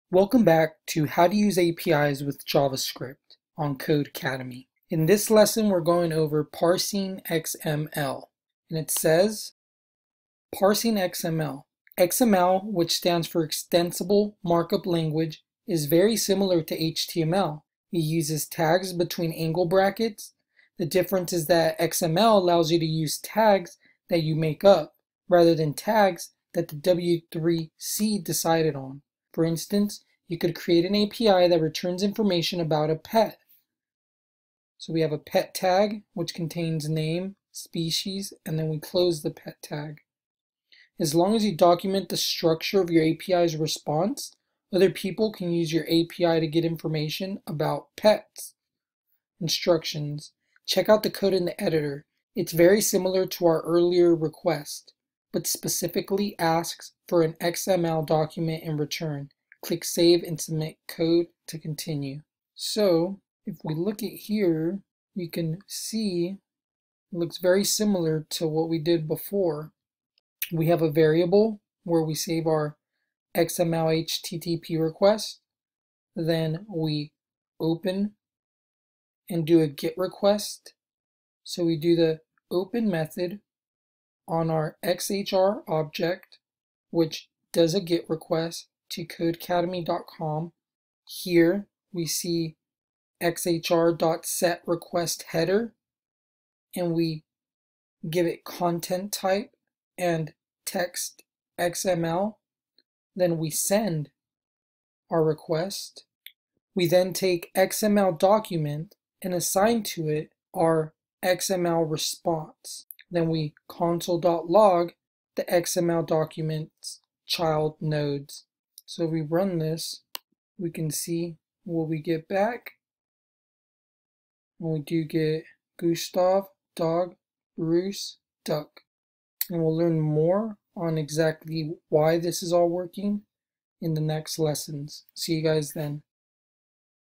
Parsing XML, Codecademy, How to use APIs with JavaScript
In this lesson we go over parsing xml with javascript.  We make a get request to codecademy and parse xml that is received in the response.  This is then printed out in a console.log